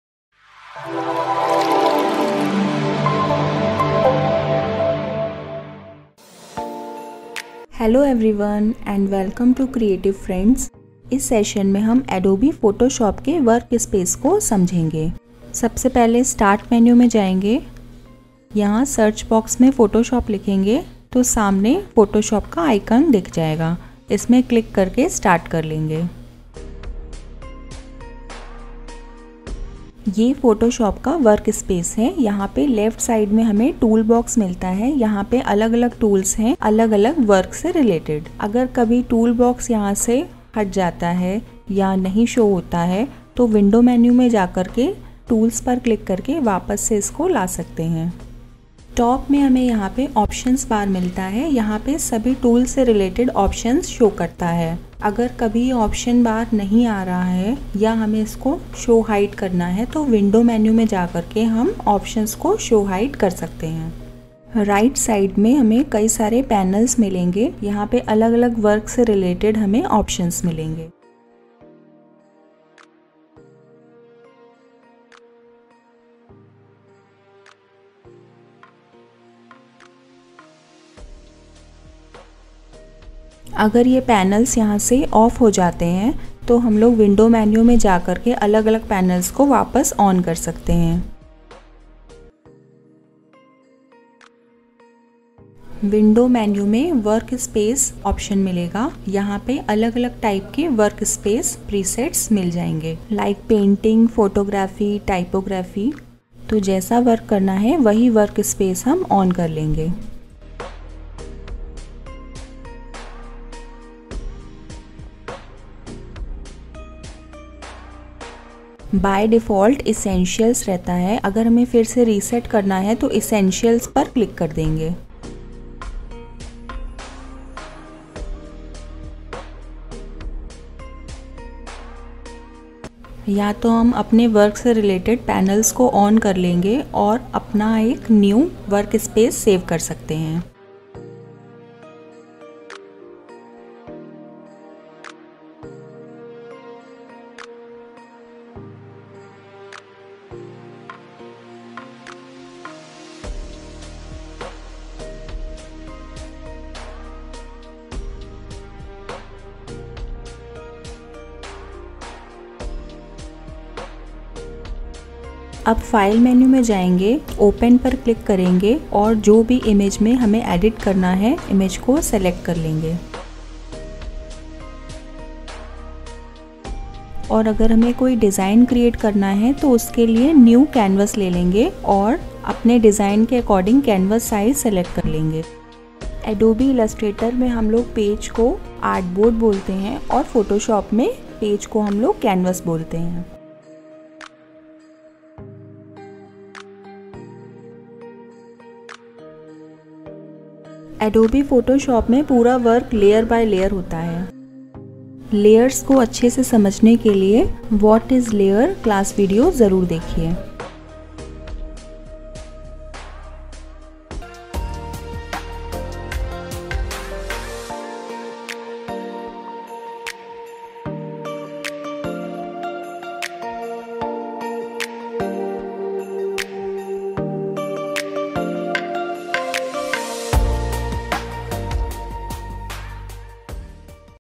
Adobe Photoshop | Beginner's Guide- Session-2 | Workspace | By Rekha Ma'am
What is Adobe Photoshop? use of Adobe Photoshop, what type of designs we can create on it.

Learning in easy way.